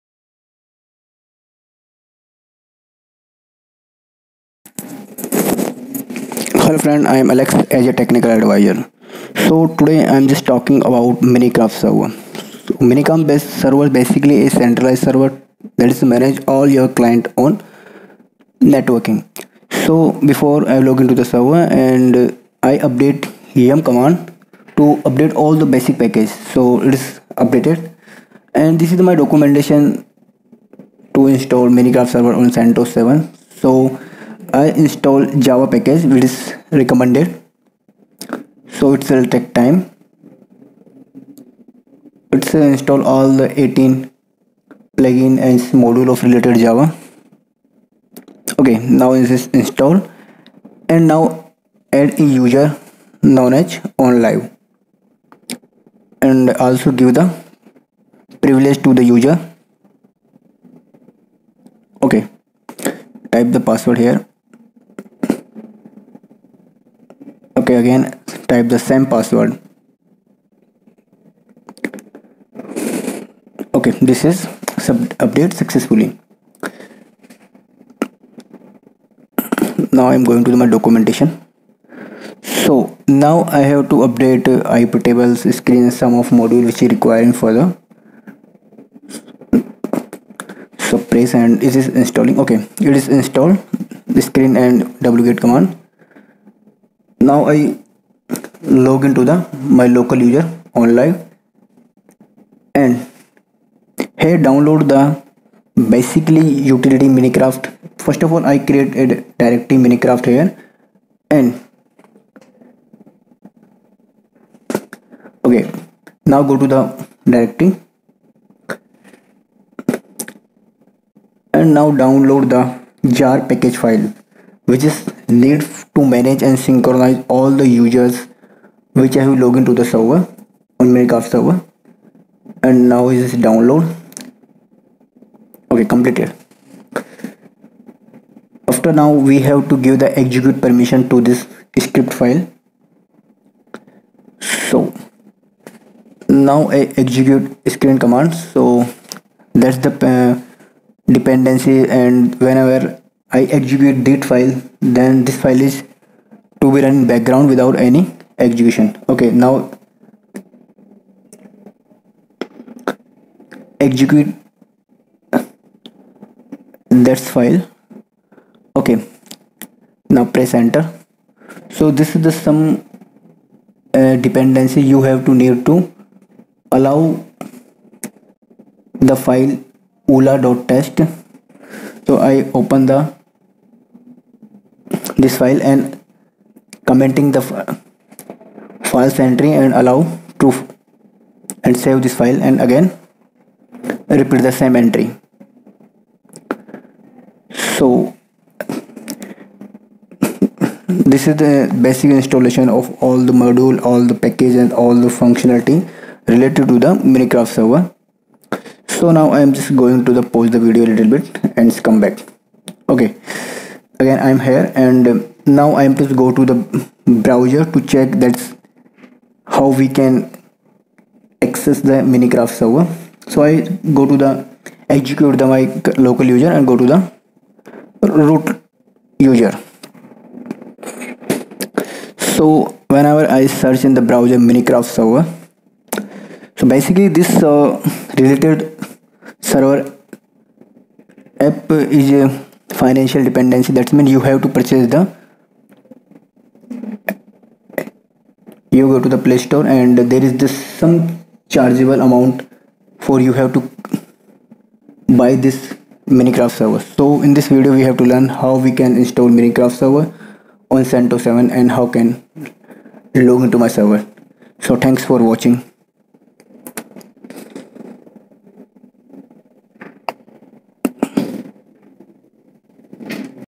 How to Build/Setup Minecraft Server on CentOS 7 - Onlive Server
In this video tutorial, we will guide you about How to Build/Setup Minecraft Server on CentOS7. Minecraft is a most popular sever site and suitable for games on streaming sites such as Twitch. While players can play the game on their own. To enjoy multiplayer mode, players need to either set up their own Minecraft server. In this tutorial you will focus on how to set up your own Minecraft server on Linux and Windows.